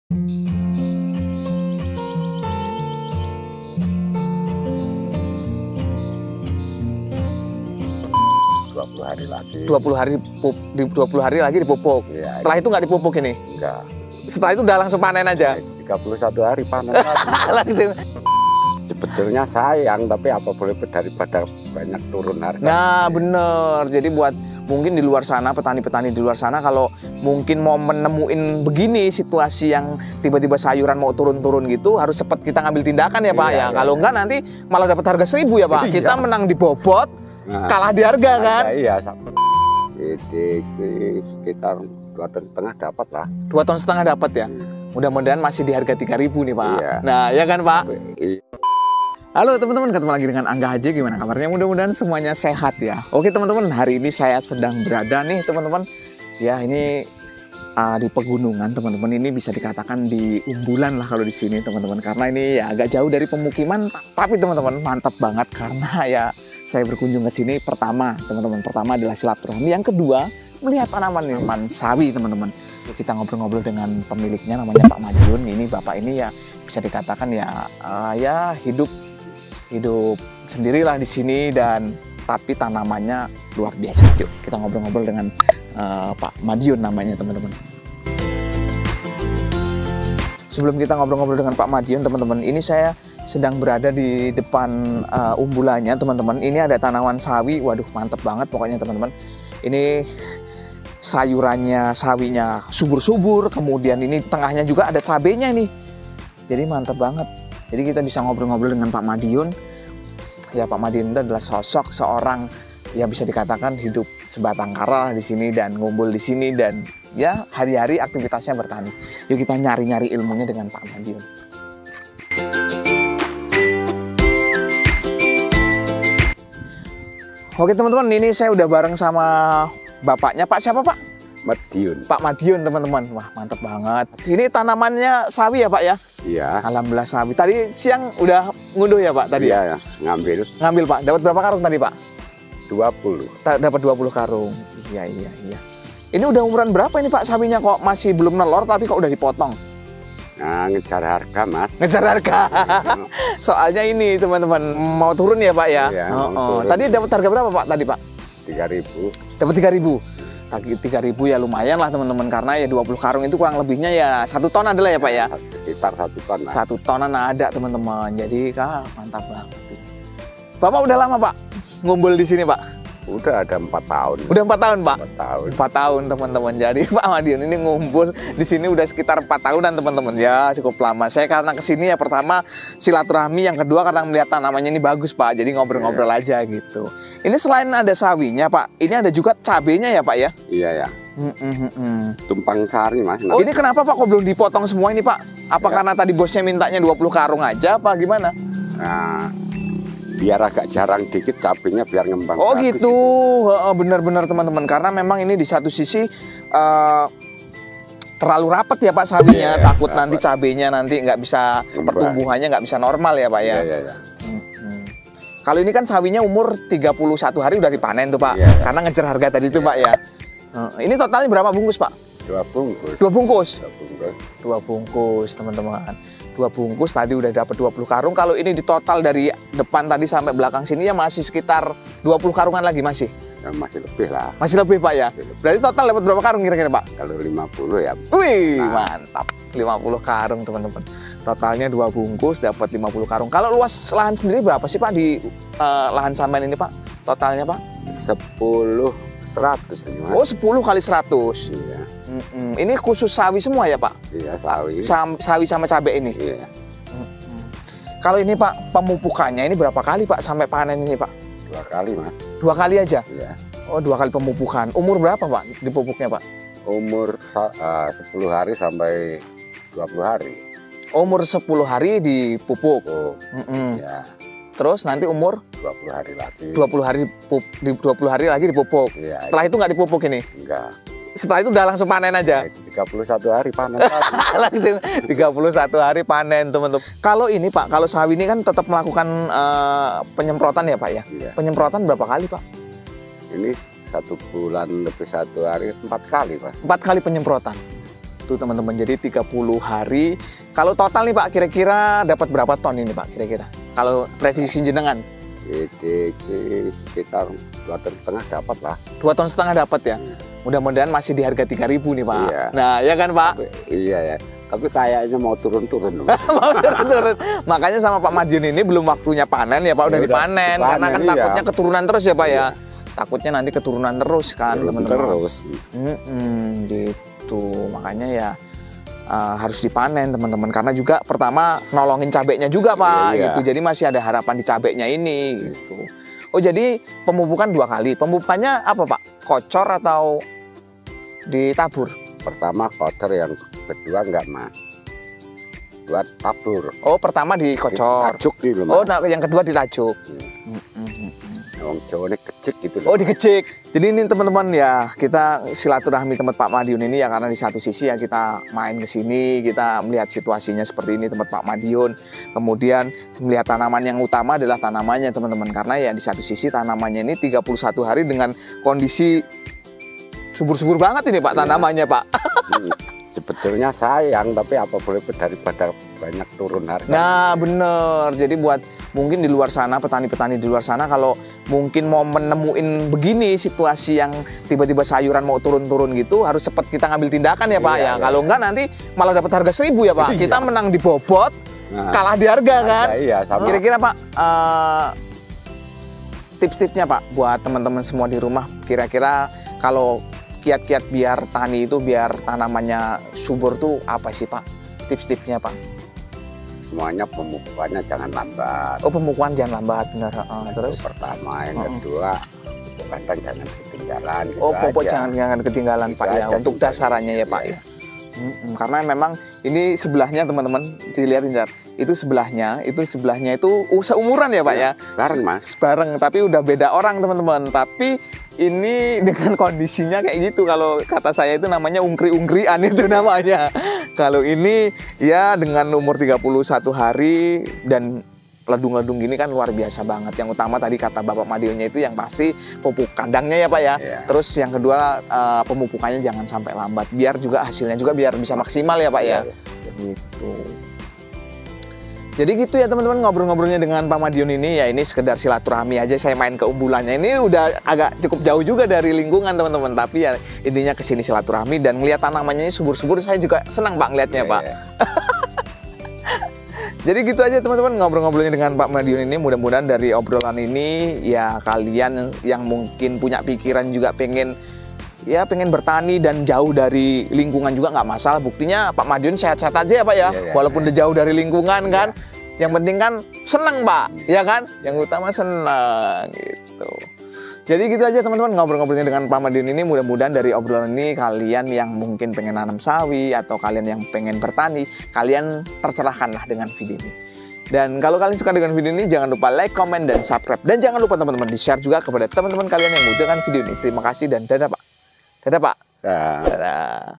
Petani Sayur 31 Hari Cuan Jutaan,Walaupun Belum Waktunya Dipanen
Hallo Gaes gimana kabarnya,mudah2han semuanya sehat ya,oke Gaes di video kali ini saya akan berkunjung ke umbulannya pak Madiun yg sukses menanam sayuran. walaupun pak Madiun tinggal di gubuk sederhana tapi tidak mengurangi semangatnya untuk bertani dan bisa mendapatkan keuntungan jutaan rupiah.
Semoga semangat bertaninya menular ya pak😊😊

INFO TANAMAN
Temuin angga HJ
Yang ingin bertanya tentang Bisnis Hasil Pertanian,Lanjut di sini ya,👇👇